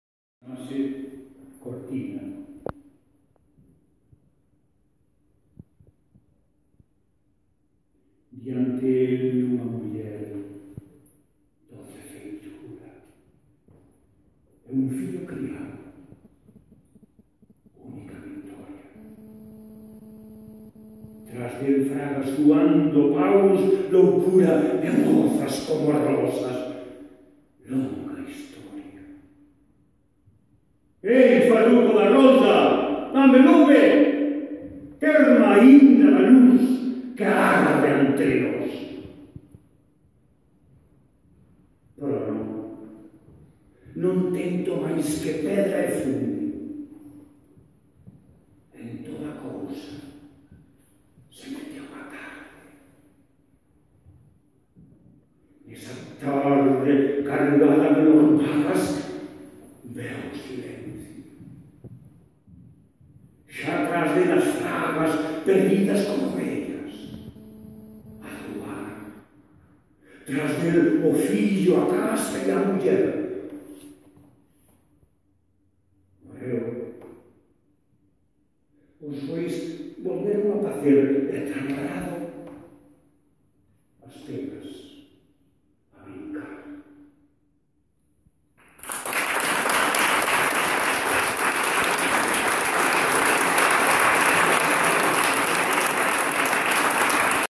'Cortina' en lembranza de Xosé María Díaz Castro
O poeta e profesor Xosé Otero Canto recita 'Cortina' de Xosé María Díaz Castro no acto do 26 aniversario do seu pasamento, celebrado na igrexa de San Xoán de Guitiriz e supón o inicio da campaña 'Salvemos o rural. Pon unha pedra na casa de Díaz Castro'. Esta é unha campaña que organiza Xermolos e a Irmandade Manuel María. No acto houbo música e poesía.